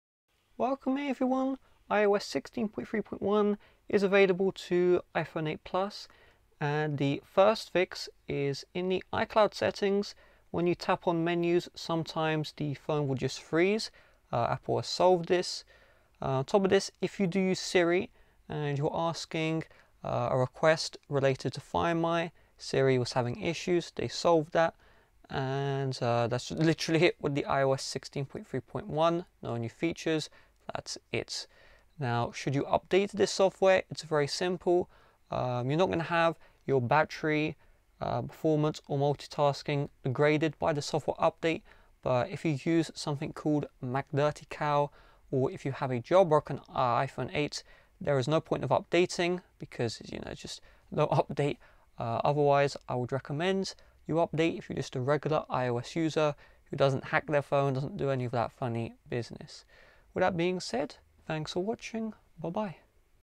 iOS 16.3.1 iPhone 8 Plus FULL REVIEW
Learn what's new with iOS 16.3.1, and whether the iOS 16.3.1 software update is worth it on iPhone 8 Plus